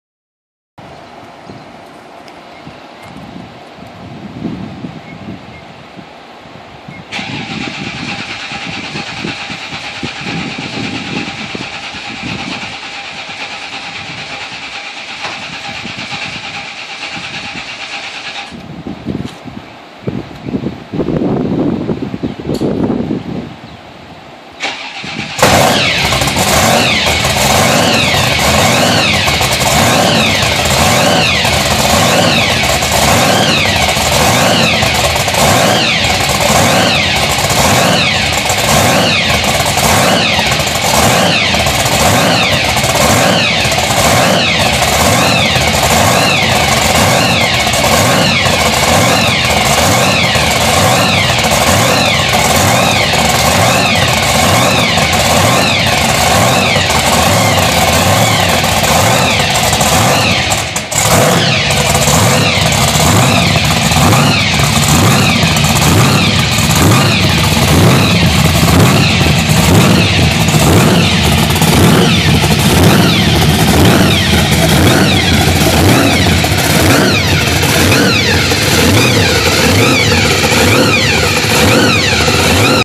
Blown and Alcohol Injected 1972 Camaro backing out of trailer
My Uncle Neil firing up his 72 Camaro and backing it out of the trailer.